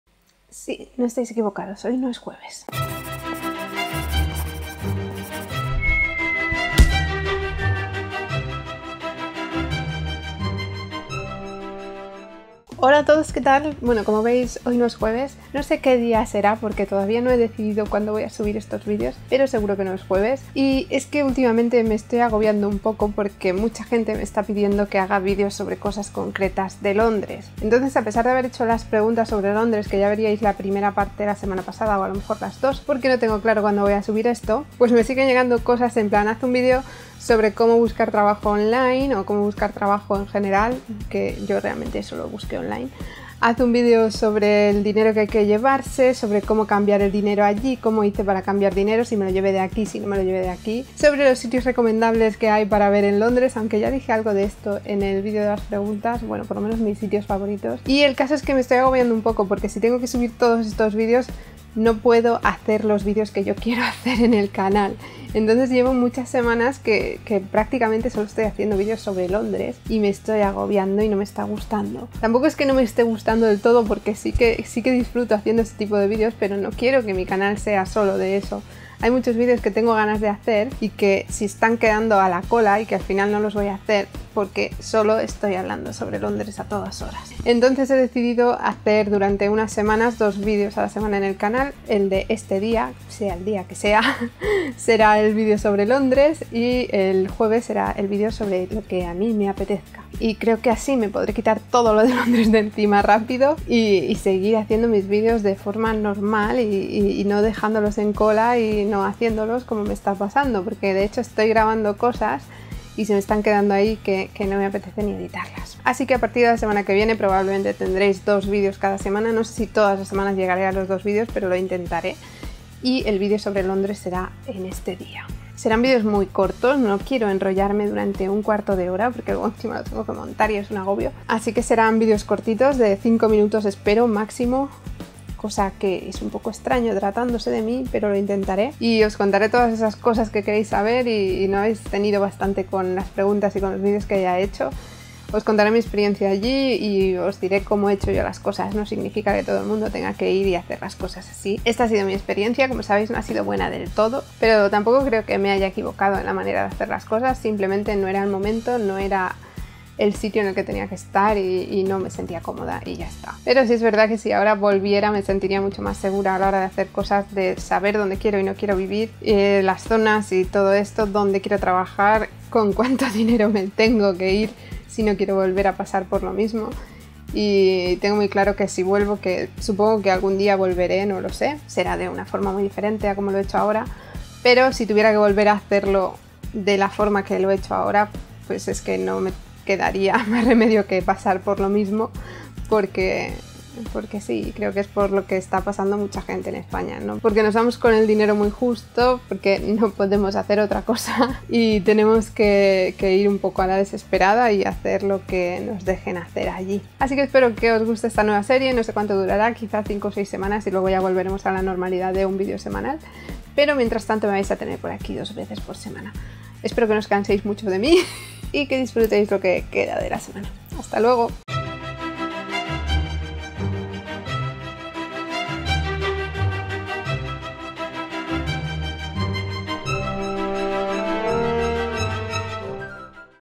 Más vídeos por semana
Pues si, a partir de esta semana tendréis un vídeo más en el canal durante un par de meses (quizá un poco más, quizá un poco menos), hasta que termine todos los vídeos de dudas sobre Londres.

Mi equipo de youtube
★ Canon EOS 100D
★ Canon 35mm
★ Grabadora Zoom H1
★ Micro de solapa JK
★ Rode Video Micro